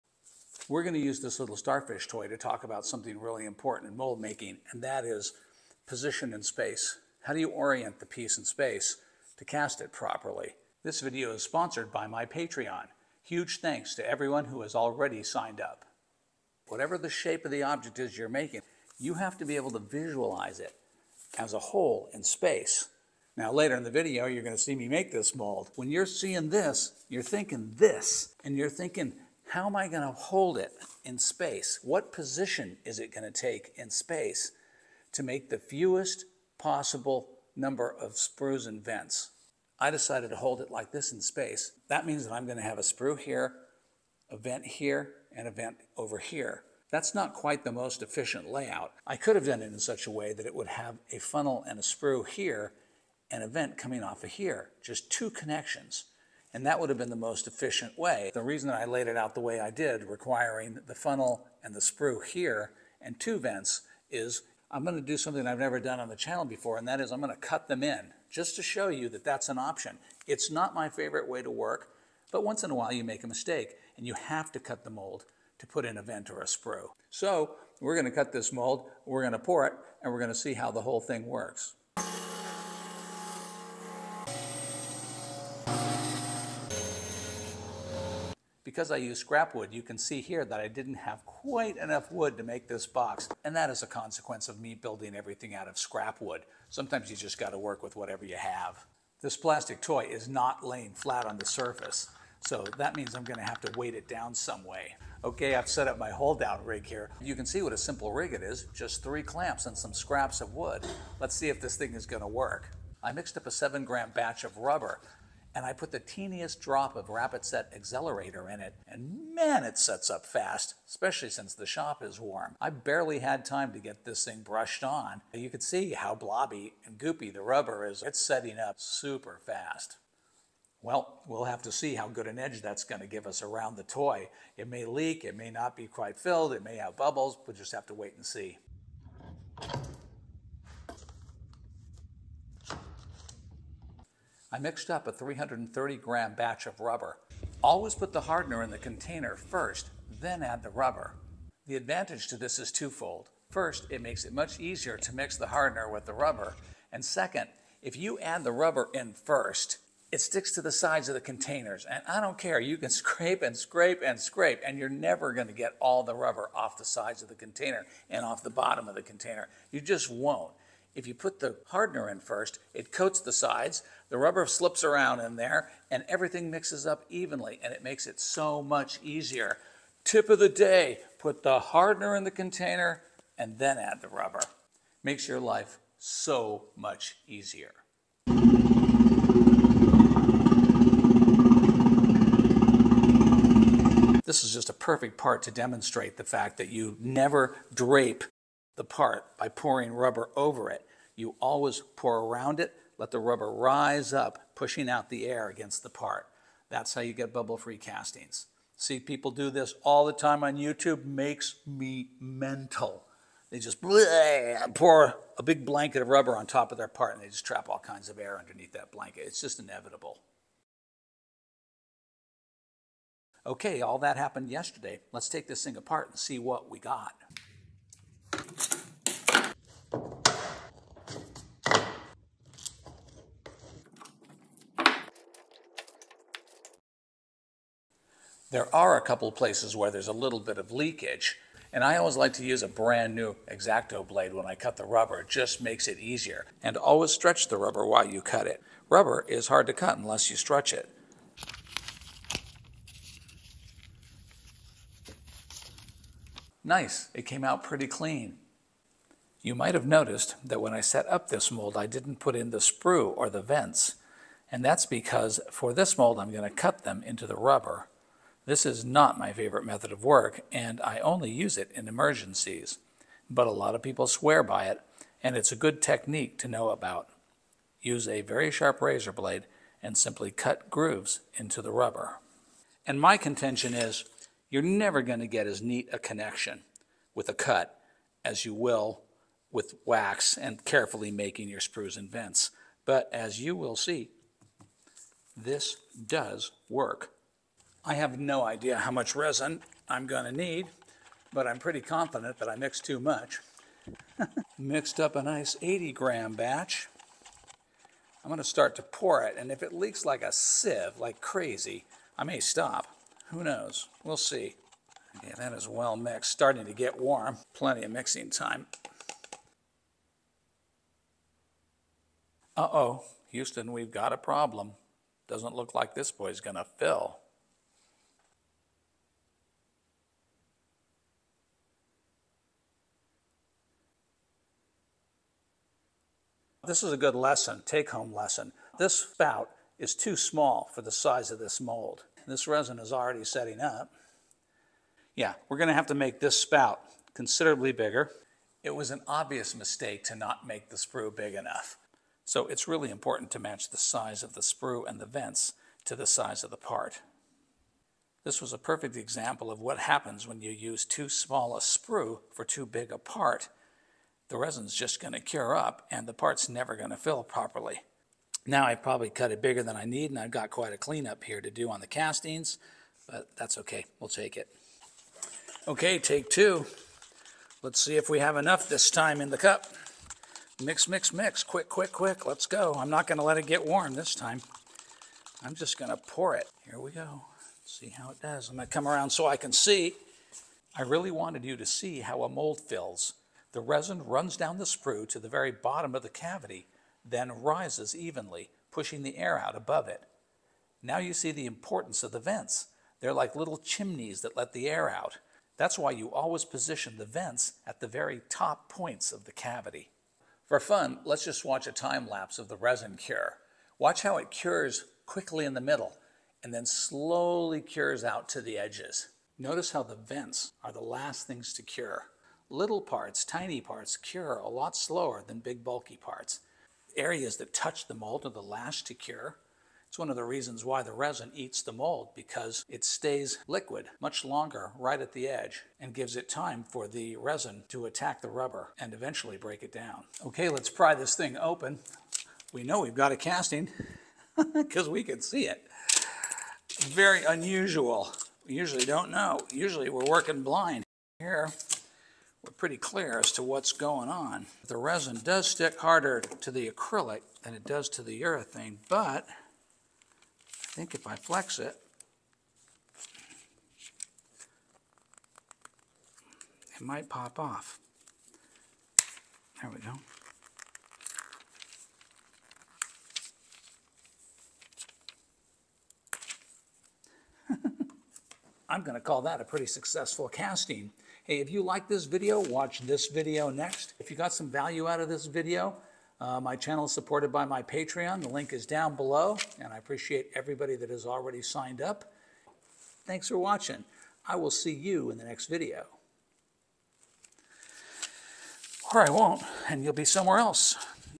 No Matter What Position, Size Matters
Getting a mold cavity to fill properly requires doing several things right. The cavity must be correctly positioned in space and it must be properly sprued and vented. This allows the resin to flow into the cavity, rise up and fill it without trapping pockets of air.